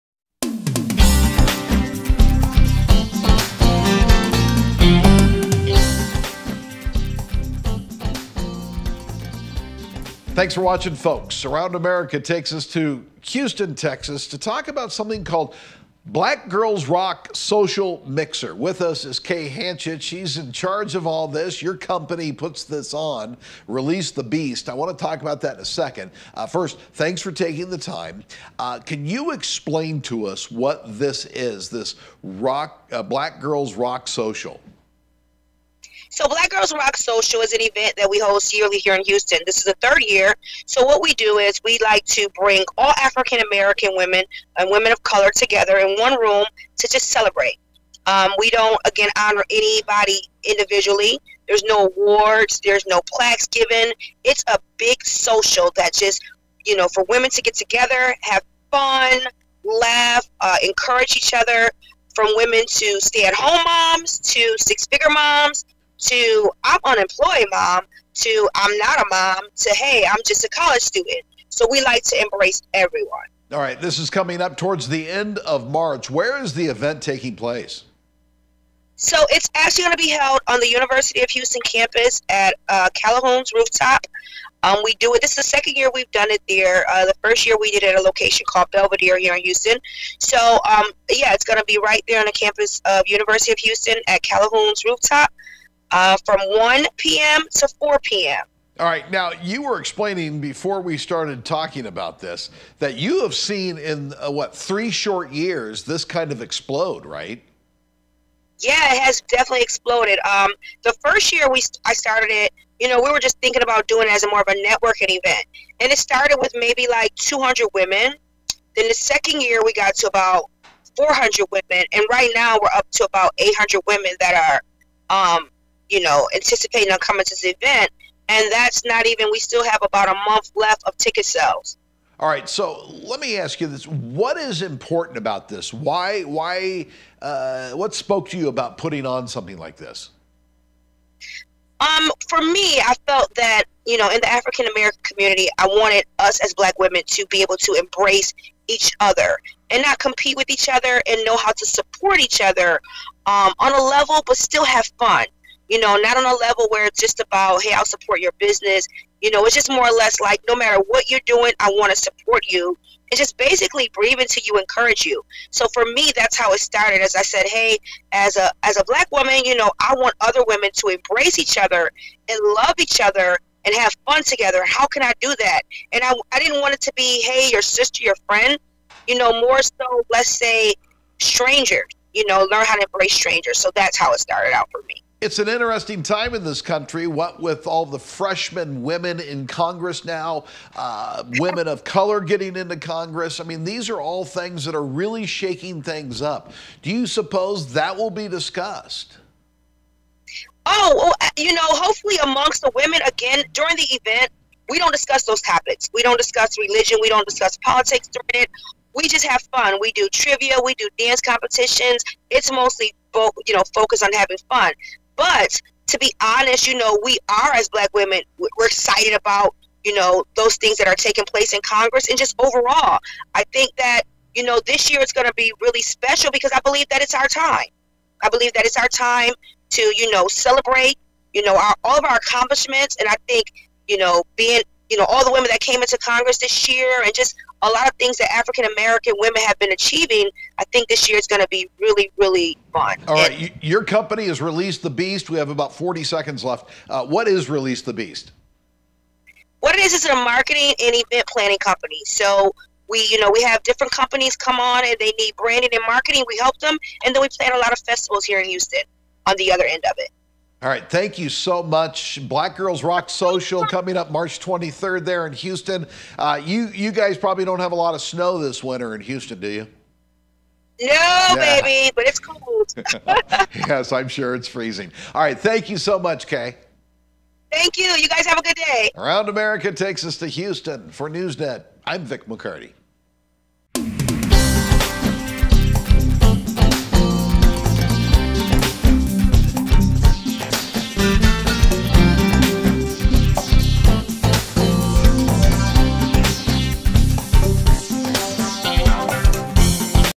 Black Girls Rock Social
We Go Around America to learn about the Black Girls Rock Social Mixer Event.Vic McCarty spoke with Kay Hatchett to learn what to expect at the event.